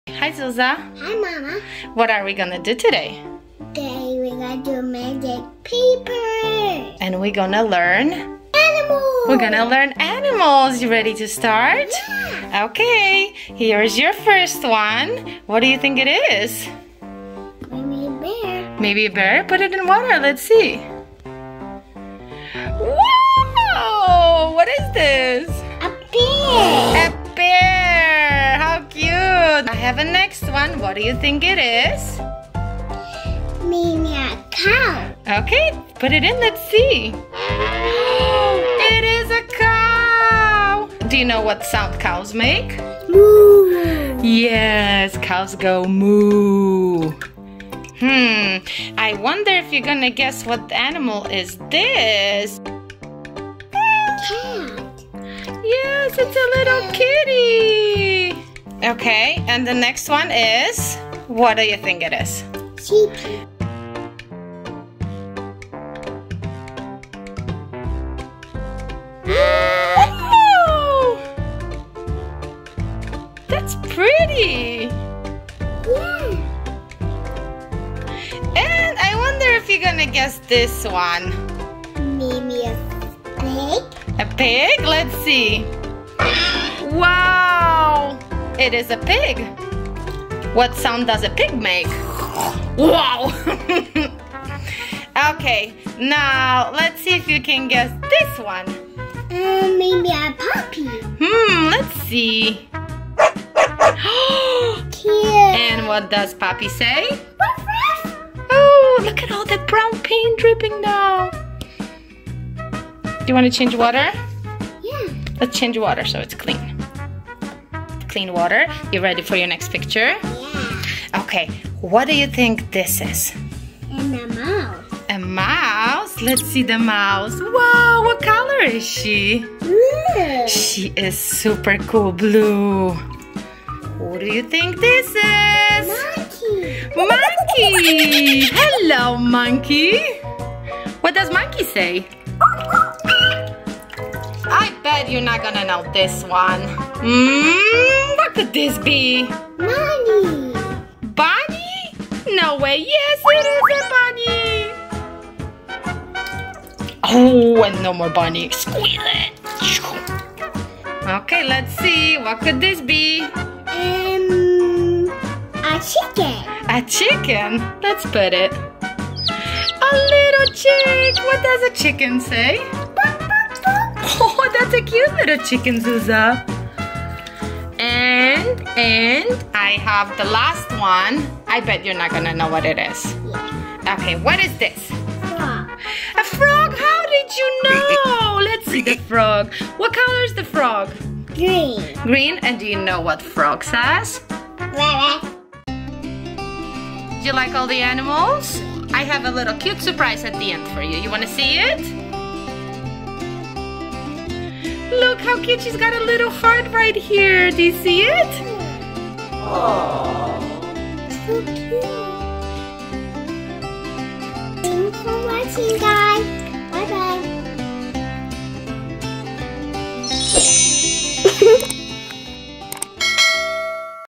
Paper Towel Magic Trick Easy DIY Science Experiments For Kids With Zuza | Learn Animals Video
Paper Towel Magic Trick is a fun easy kids activity to do together at home !! Today we are learning animals with paper towel drawings and water !